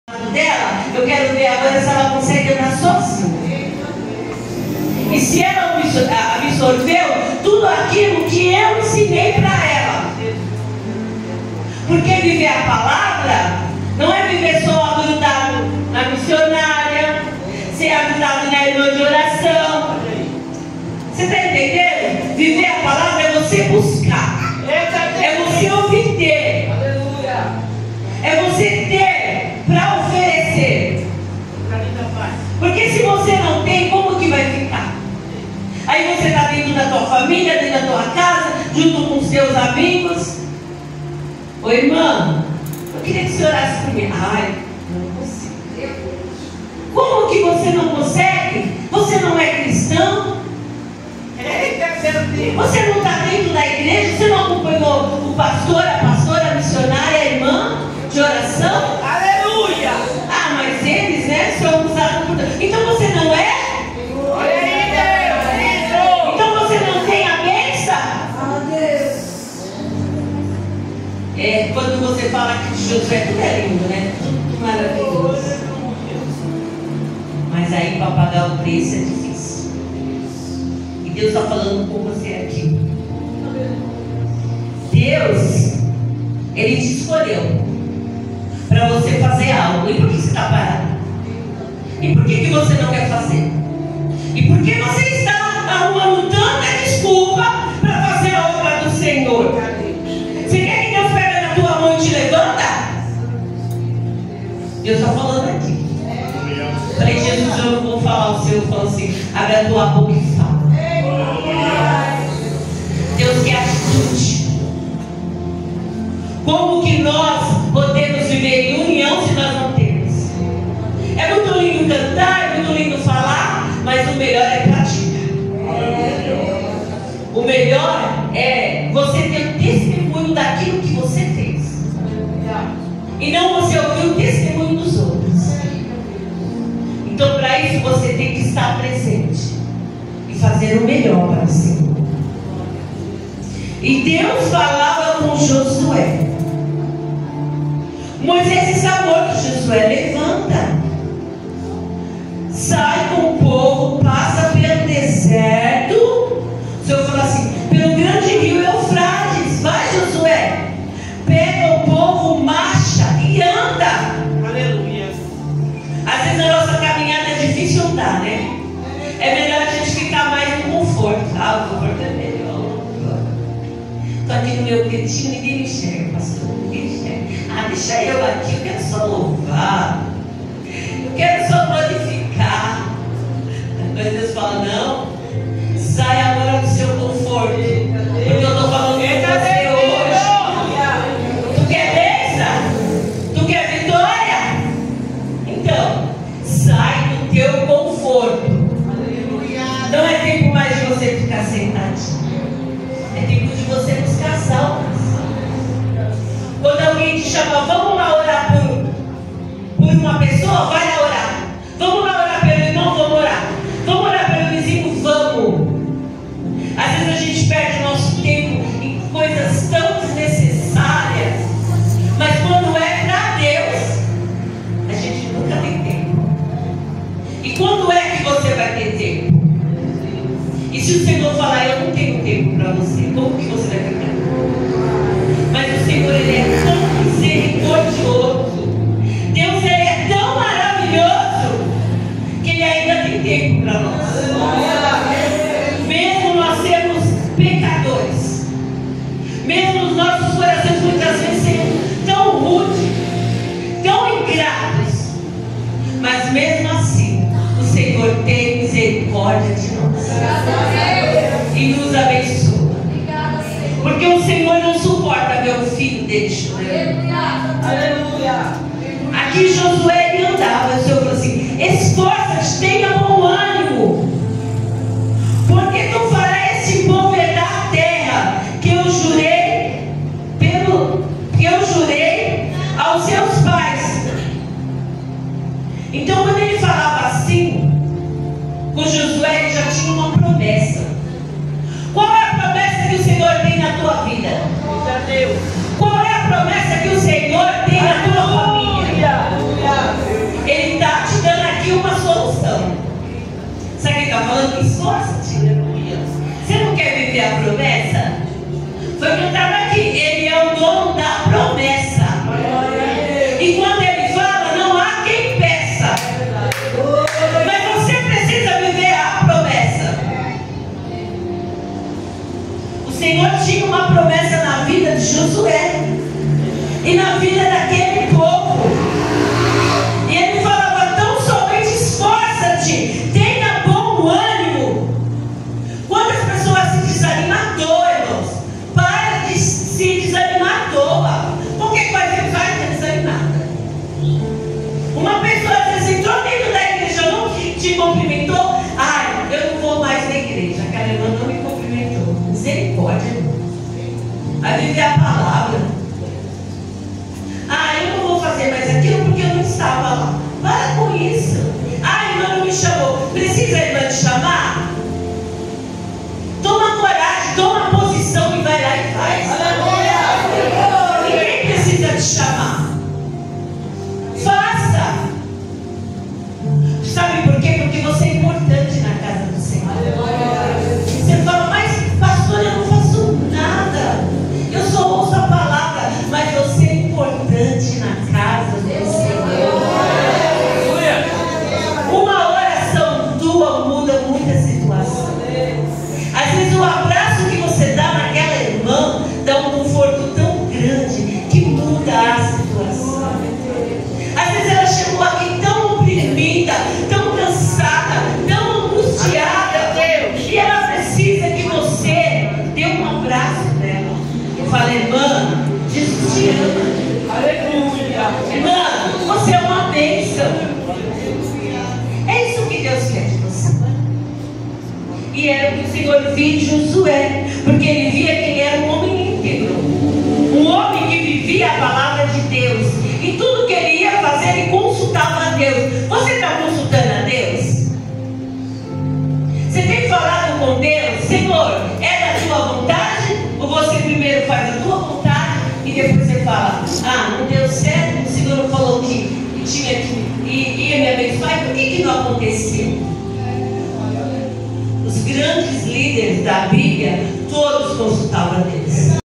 I P V C , OUTUBRO ROSA MINISTRANDO A PASTORA LUIZA. 12/10/2024.
CULTO DE MULHERES, OUTUBRO ROSA, I P V C MINISTRANDO A PALAVRA A PASTORA LUIZA.